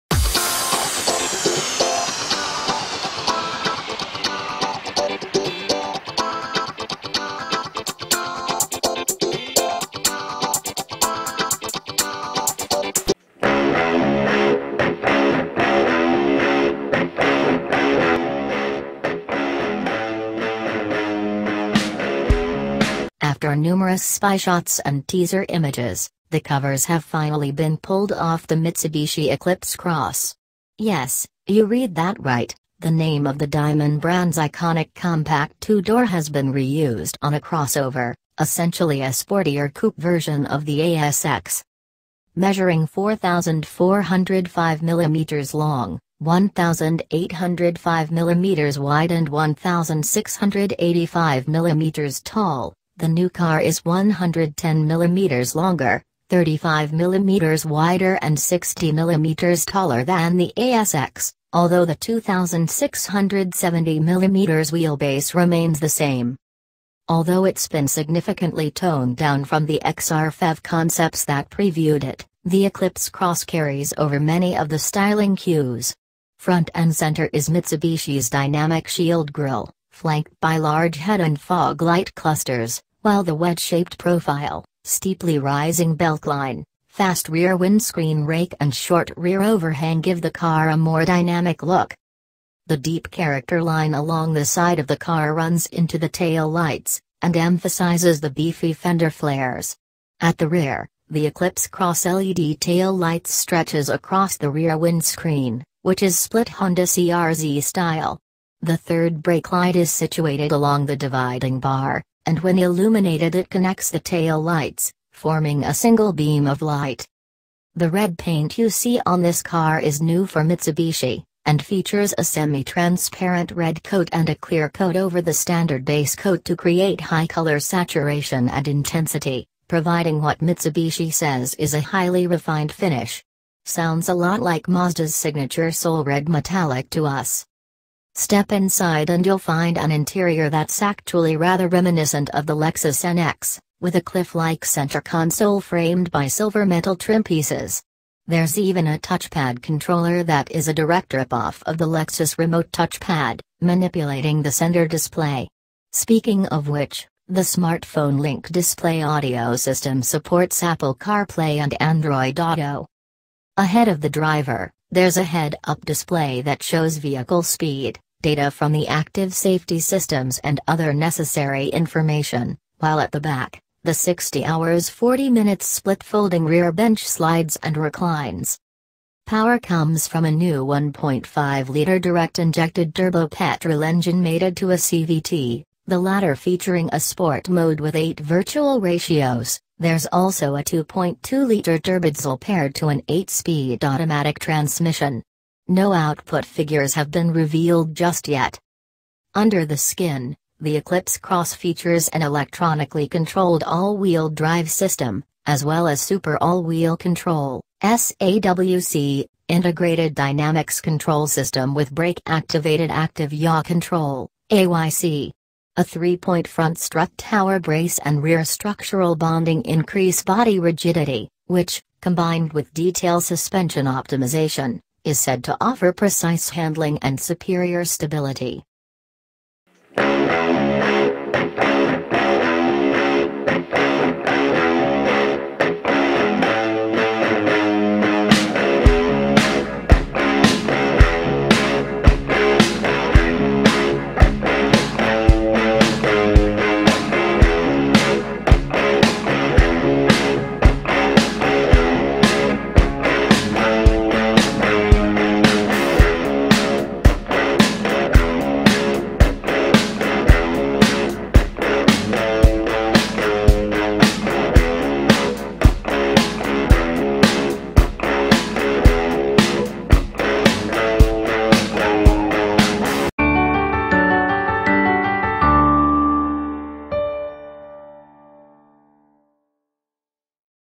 LOOK - MITSUBISHI ECLIPSE CROSS 2017
"MITSUBISHI ECLIPSE CROSS 2017 "After numerous spyshots and teaser images, the covers have finally been pulled off the Mitsubishi Eclipse Cross. Yes, you read that right – the name of the diamond brand’s iconic compact two-door has been reused on a crossover, essentially a sportier “coupé” version of the ASX.